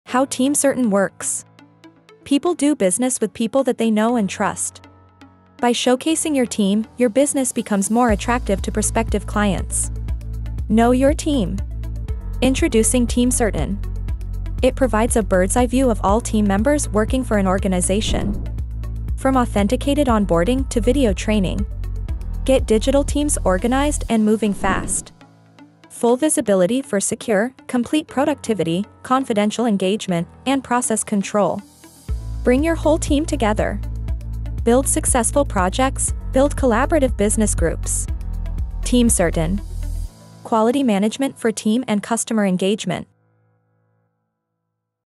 How TeamCERTin Works
In this video tutorial, we will introduce you to TeamCERTin, the secure and reliable platform that brings your team together.

We'll cover the following:

 Introduction to TeamCERTin.
 Benefits of Using TeamCERTin.

By the end of this video, you will know the benefits and unique features of TeamCERTin. Build successful projects and collaborative business groups with TeamCERTin.